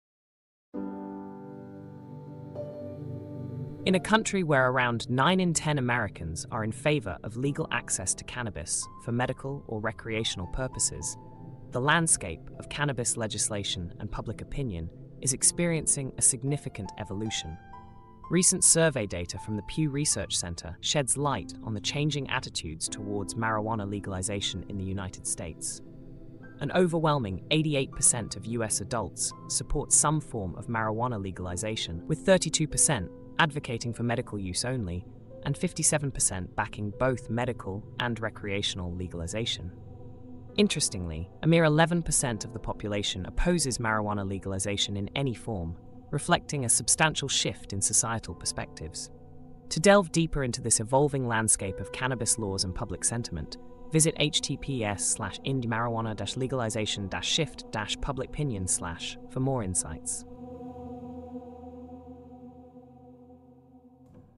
The Evolution of Marijuana Legalization in the U S
Discover the shifting landscape of cannabis laws and public opinion in America with insights from the Pew Research Center.